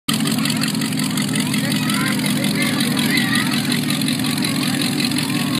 Big Festival Foods Shopping Cart. Christmas Parade!!
Come every November night before Thanksgiving. Best parade in Manitowoc in the County!!